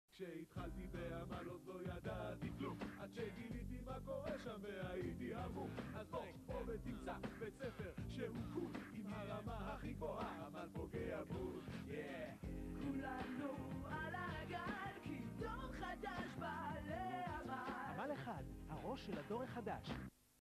Music Maker CD ROM Commercial MTV Gadash 1997
The language in this piece is foreign to me so, I hope someone will help me to find the name of this product. Then I can change the title and tags in order for more people to find this ad.